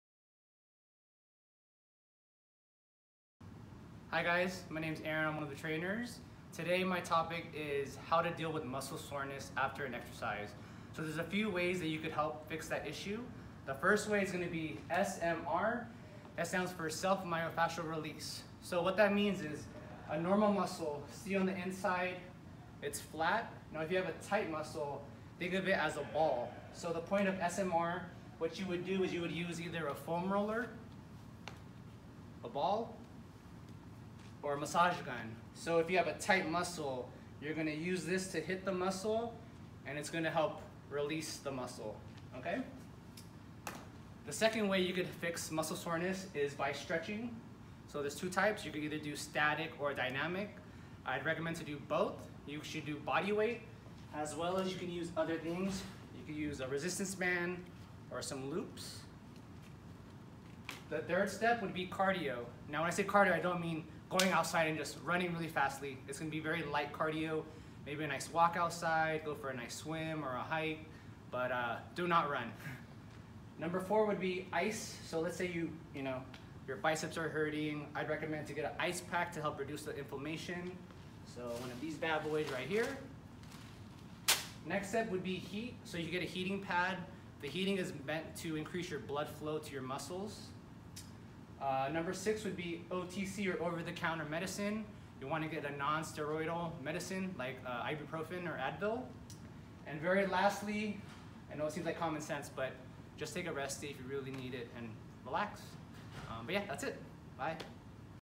How to deal with muscle soreness from exercise
Our amazing trainer, Aaron gives us some tips on how to get some recovery for muscle soreness from exercise.